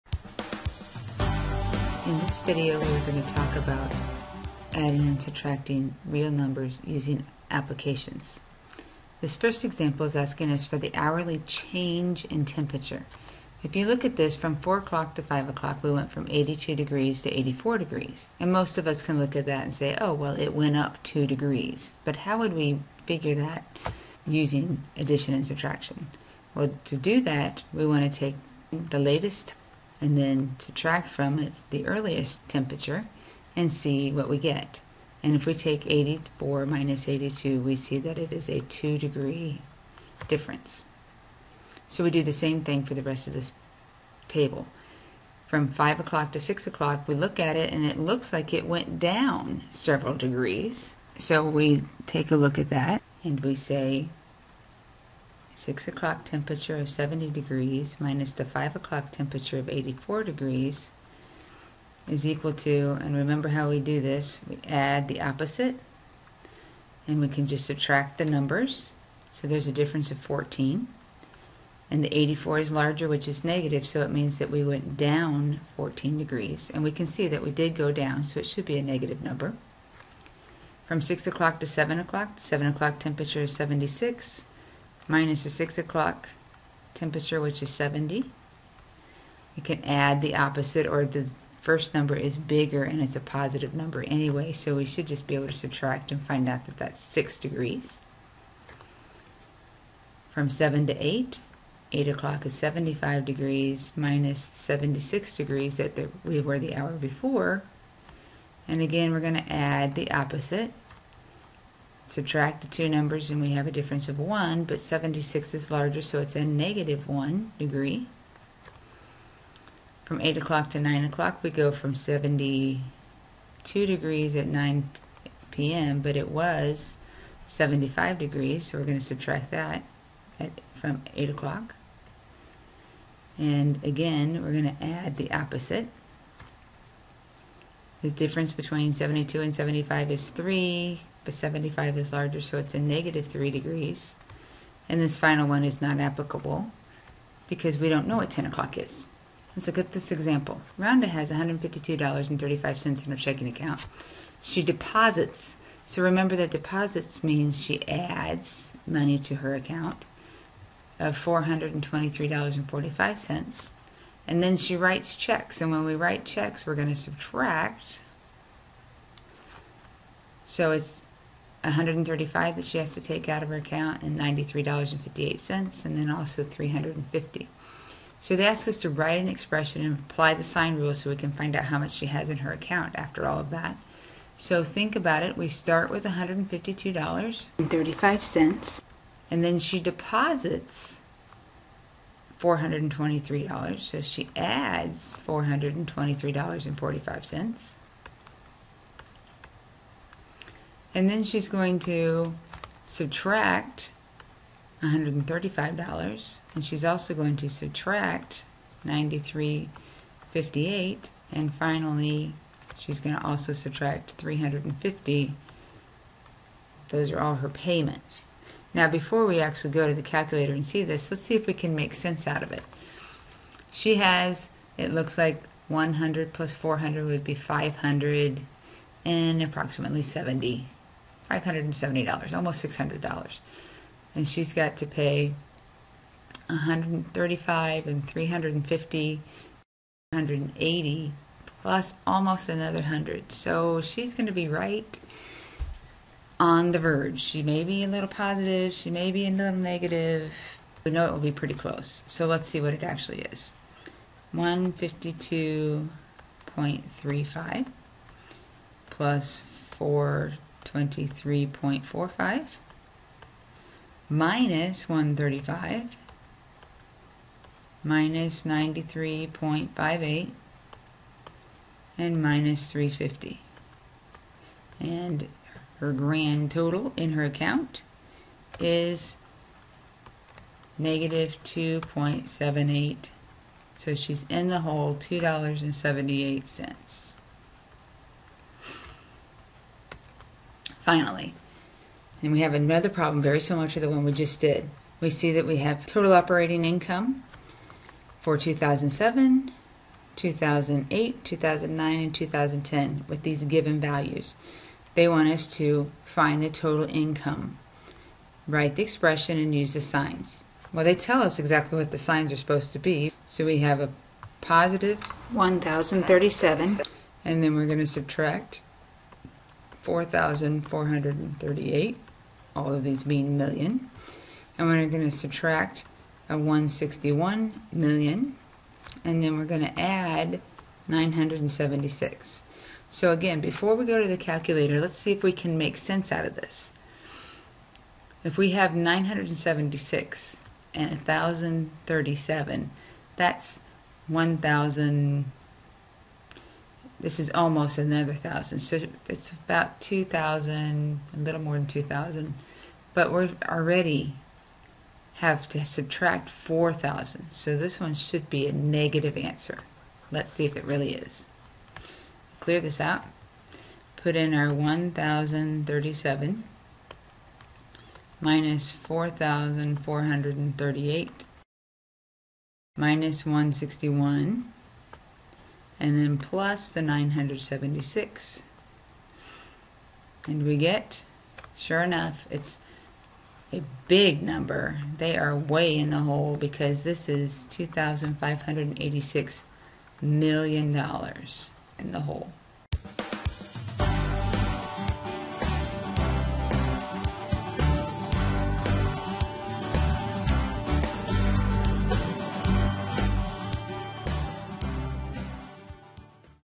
097- 9 ADD and SUBTRACT REALS-APPLICATIONS
Shows how to use the sign rules in applications